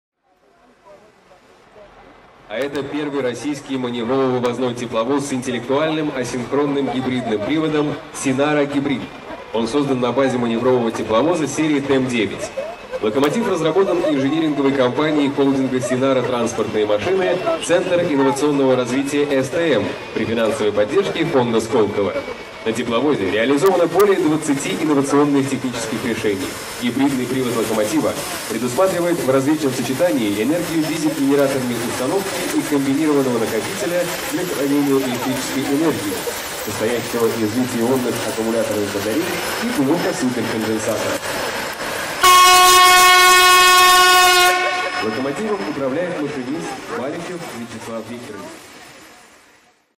Тепловоз ТЭМ9H-001 "SinaraHybrid" на I\/ Международном ж/д салоне "ЭКСПО 1520" в Щербинке.
Снято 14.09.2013 г. на ЭК ВНИИЖТ в Щербинке. Московская область, Курское направление Московской ж/д, 33 км от Курского вокзала. Маневровый тепловоз ТЭМ9H-001 "SinaraHybrid" участвует в динамической экспозиции. Видео снято фотоаппаратом Nikon COOLPIX s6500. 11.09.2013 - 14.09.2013 г.г. проходил I\/ Международный железнодорожный салон техники и технологий "ЭКСПО 1520". Тепловоз ТЭМ9H-001 - Тепловоз с Электрической передачей, Маневровый, тип-9, H - гибридный, 001 - порядковый номер. Построен: в 2012 г. Завод-изготовитель: Людиновский тепловозостроительный завод, г. Людиново Калужской области, Россия. Дорога приписки: Московская железная дорога. Депо: ОАО "Всероссийский НИИ железнодорожного транспорта". Серия: ТЭМ9H. Состояние: новый (не был в регулярной эксплуатации).